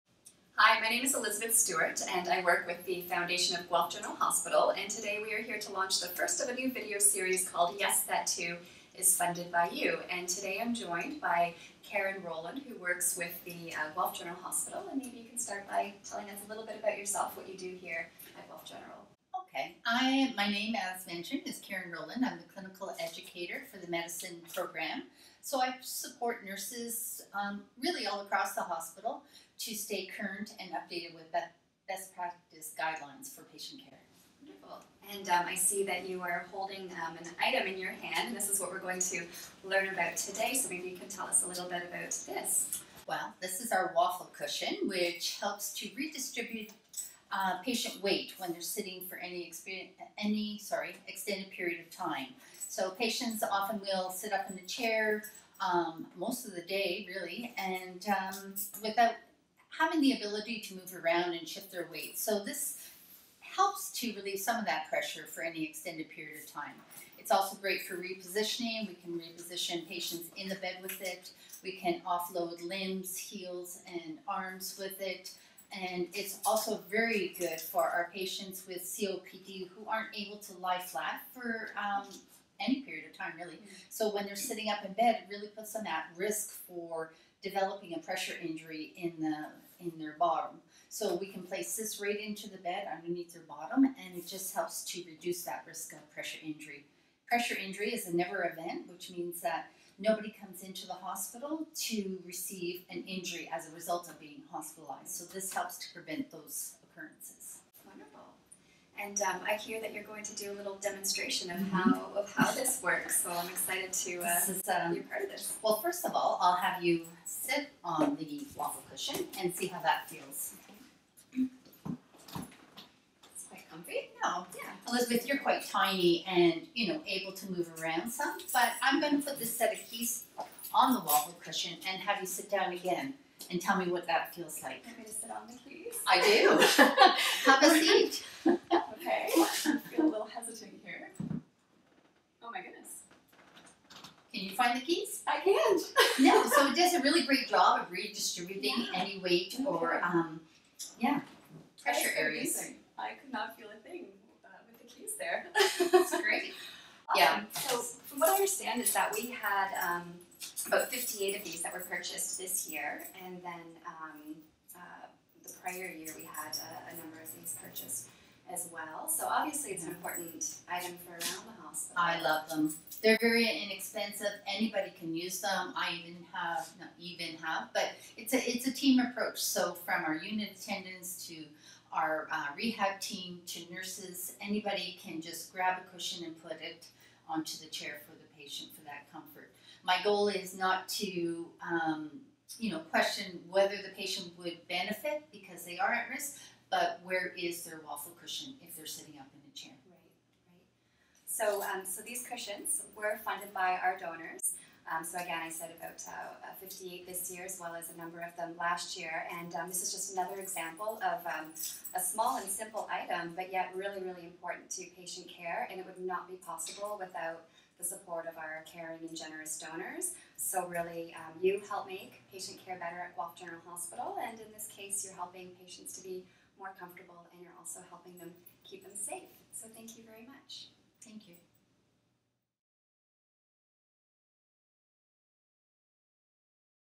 Yes, That Too is Funded by You - Waffle Cushion
Elizabeth Stewart from The Foundation of Guelph General Hospital talks with Karen Rolland from Guelph General Hospital about a small but meaningful patient care tool that our kind donors help fund.